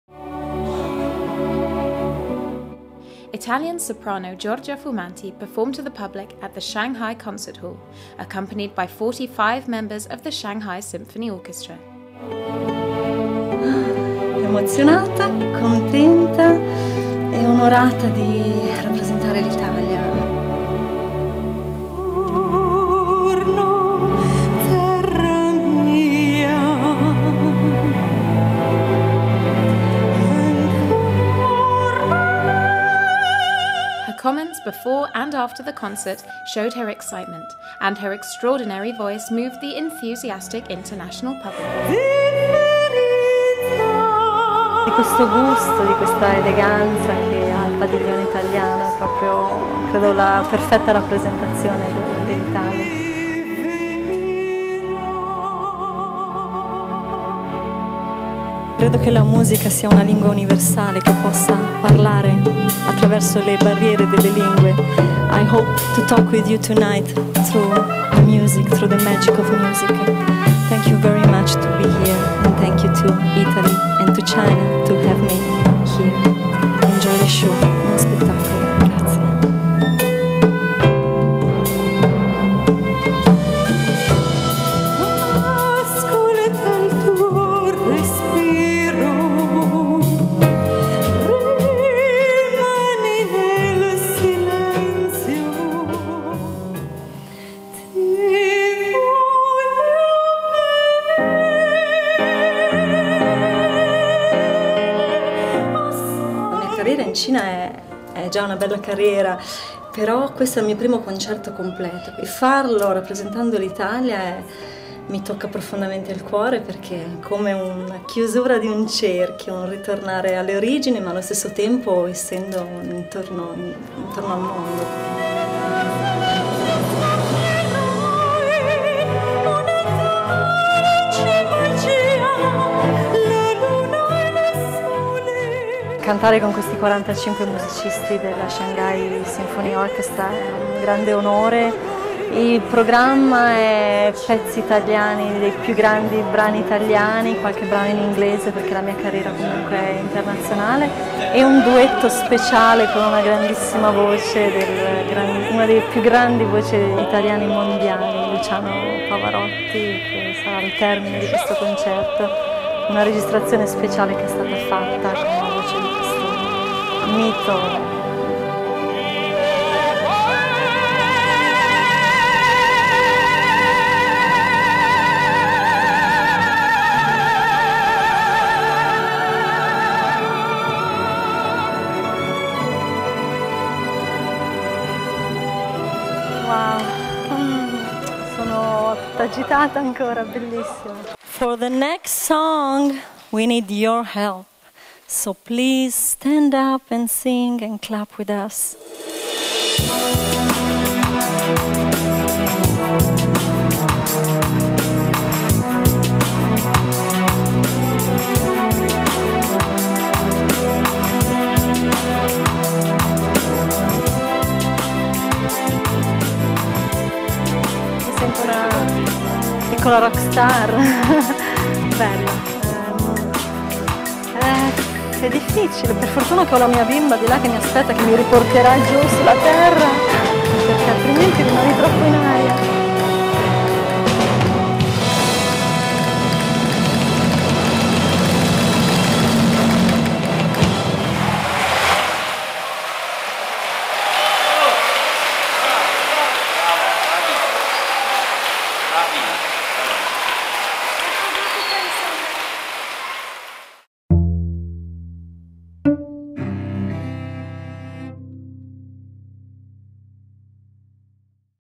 Giorgia Fumanti Concert at the Shanghai Concert Hall
On the 26th July Italian soprano Giorgia Fumanti performed at the Shanghai Concert Hall accompanied by 45 members of the Shanghai symphony orchestra. An exciting show with Italian and English pieces and a suprise finale!